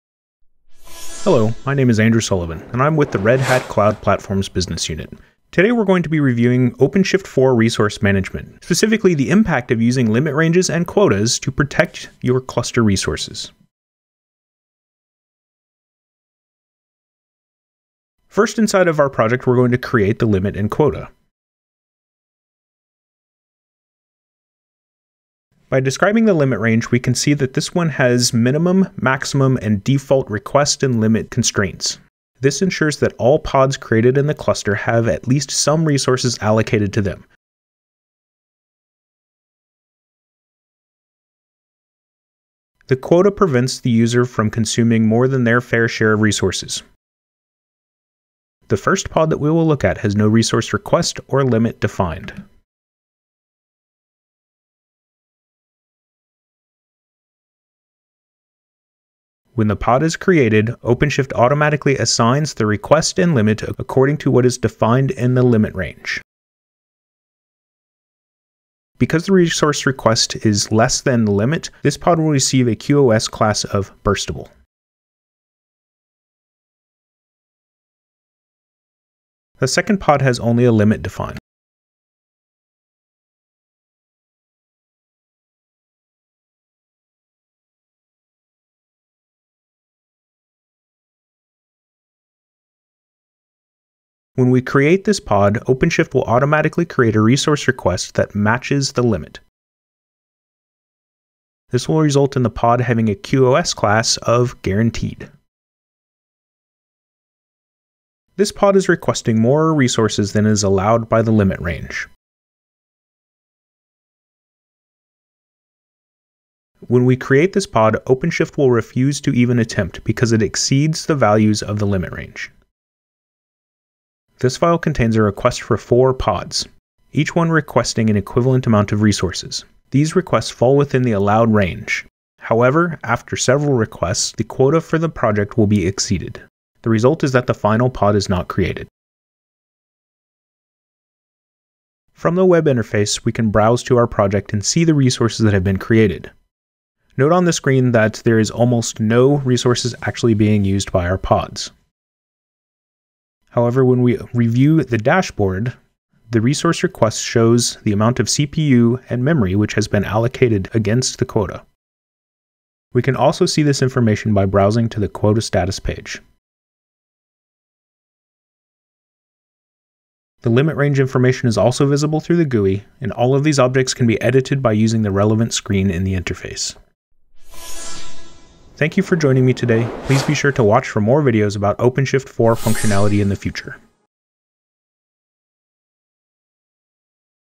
OpenShift 4 Resource Management
Controlling the resources being used by pods and projects is an important aspect of cluster management.  Ensuring that pods receive their "fair share" of resources and that noisey neighbors do not adversely affect peers is important to overall success of the OpenShift cluster and the applications.  This video demonstrates using limit ranges and quotas to enact controls on resource consumption by pods.